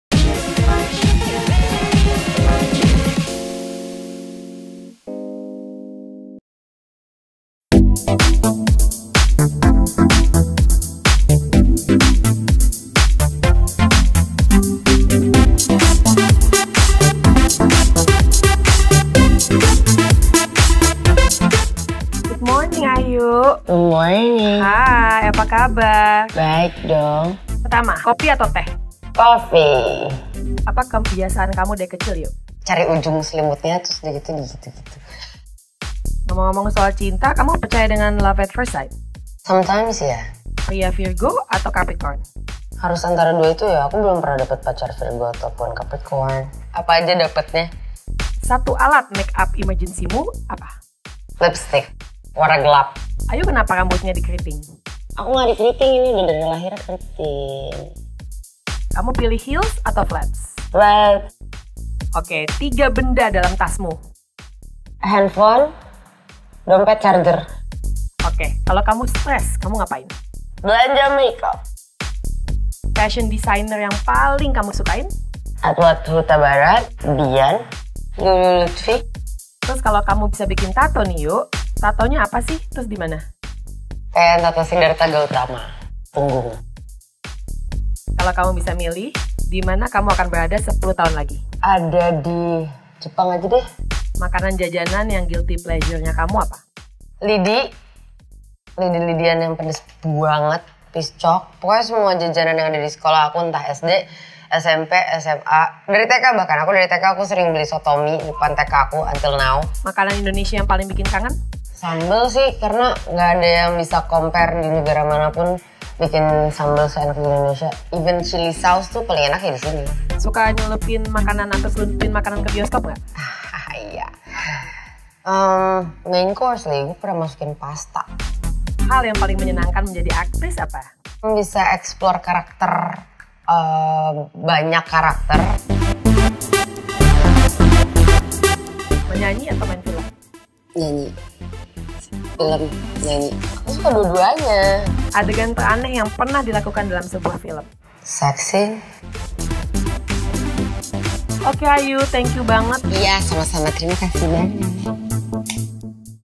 Impromptu Questions With Ayushita
Apakah Ayushita percaya dengan love at first sight? Dan apa yang ia lakukan kalau sedang stress? Ayushita (What They Don’t Talk When They Talk About Love) menjawab pertanyaan-pertanyaan dari SLATE saat bersiap untuk photoshoot.